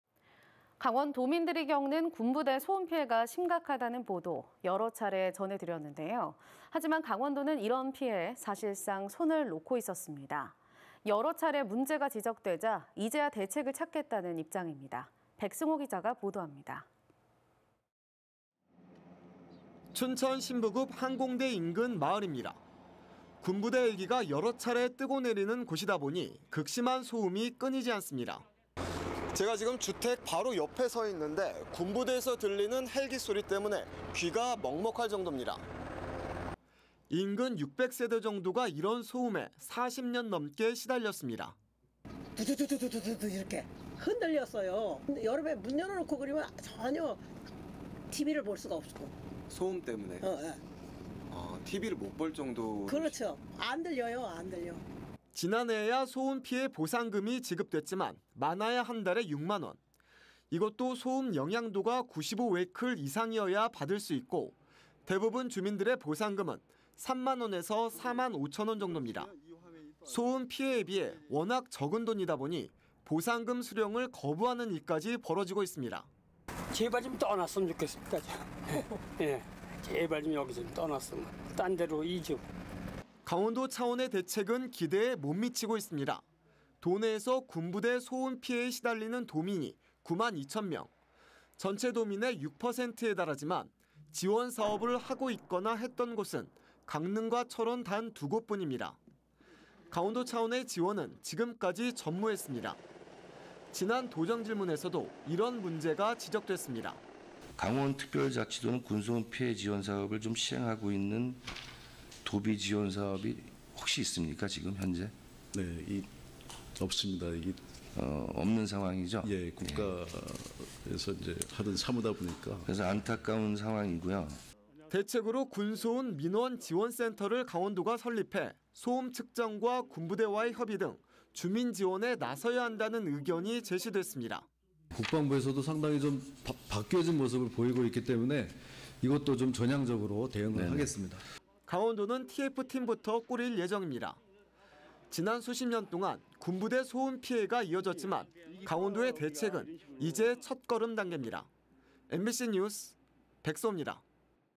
여전한 군 소음 피해.. 강원도는 뭐하나?
강원도민들이 겪는
군부대 소음 피해가 심각하다는 보도
여러 차례 전해드렸습니다.

 하지만 강원도는 이런 피해에
사실상 손을 놓고 있었습니다.

 여러 차례 문제가 지적되자 이제야
대책을 찾겠다는 입장입니다.

 백승호 기자가 보도합니다.
춘천 신북읍 항공대 인근 마을입니다.

 군부대 헬기가 여러 차례
뜨고 내리는 곳이다 보니
극심한 소음이 끊이지 않습니다.

[백승호 기자]
"제가 지금 주택 바로 옆에 서 있는데,
군부대에서 들리는 헬기 소리 때문에
귀가 먹먹할 정도입니다."

 인근 6백 세대 정도가
이런 소음에 40년 넘게 시달렸습니다.

◀ INT ▶[심순옥/마을 주민]
"(창문이) 두두두두 이렇게 흔들렸어요.
여름에 문 열어놓고 그러면 전혀
TV를 볼 수가 없고...
(소음 때문에?) 네...
(TV를 못 볼 정도로...)
그렇죠. 안 들려요. 안 들려..."

 지난해에야 소음 피해 보상금이
지급됐지만 많아야 한 달에 6만 원.

 이것도 소음 영향도가
95웨클 이상이어야 받을 수 있고,
대부분 주민들의 보상금은
3만 원에서 4만 5천 원 정도입니다.

 소음 피해에 비해
워낙 적은 돈이다 보니 보상금 수령을
거부하는 일까지 벌어지고 있습니다.

◀ INT ▶[이우형/마을 주민]
"제발 좀 떠났으면 좋겠습니다.
제발 좀 여기서 떠났으면...
다른 곳으로 이전..."

 강원도 차원의 대책은
기대에 못 미치고 있습니다.

 도내에서 군부대 소음 피해에
시달리는 도민이 9만 2천 명.

 전체 도민의 6%에 달하지만
지원 사업을 하고 있거나 했던 곳은
강릉과 철원 단 두 곳뿐입니다.

 강원도 차원의 지원은 지금까지 전무했습니다.

 지난 도정질문에서도
이런 문제가 지적됐습니다.

◀ INT ▶[최규만/강원도의원]
"강원특별자치도는 군 소음
피해 사업을 좀 시행하고 있는
도비 지원 사업이 있습니까 현재?
(없습니다. 이게 국가에서 하는
사무다 보니까...)
안타까운 상황이고요..."

 대책으로 군소음 민원지원센터를
강원도가 설립해 소음 측정과
군부대와의 협의 등 주민 지원에
나서야 한다는 의견이 제시됐습니다.

◀ INT ▶[김진태/강원도지사]
"국방부에서도 상당히 바뀌어진 모습을
보이고 있기 때문에 이것도
전향적으로 대응하겠습니다."

 강원도는 TF팀부터 꾸릴 예정입니다.

 지난 수십 년 동안
군부대 소음 피해가 이어졌지만
강원도의 대책은 이제 첫걸음 단계입니다.

 MBC 뉴스 백승호입니다.◀ END ▶

(영상취재/이인환)